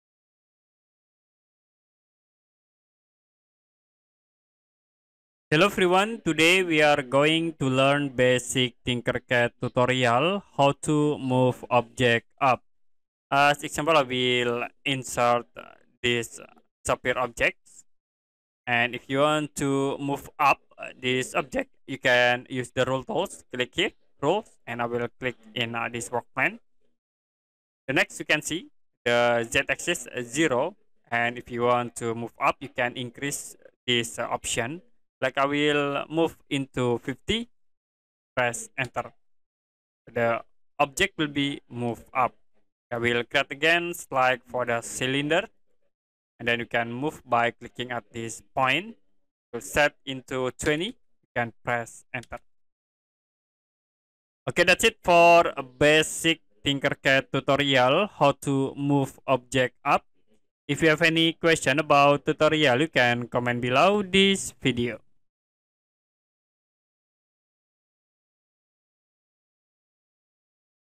TinkerCAD How To Move Object Up
TinkerCAD is a popular 3D design tool that allows users to easily move objects up, down, and in different directions. Learn how to move objects up in TinkerCAD with these simple steps.